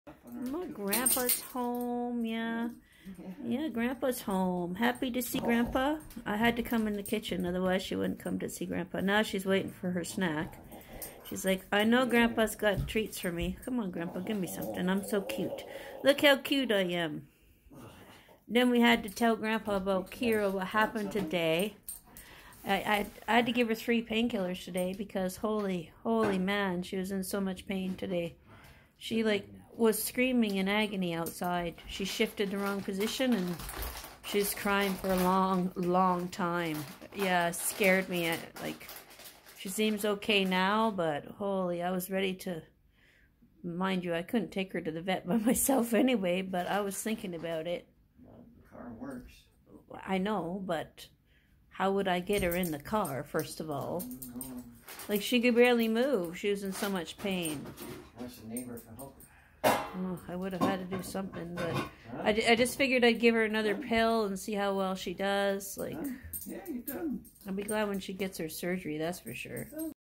🎉🐶🐱 "Keera and Daisy are over the moon!
Grandpa's finally home, and they can't contain their excitement! They know he's the treat master! 🍖🍪 Watch their adorable reaction! HappyPets GrandpaIsBack TreatTime" 🏡👴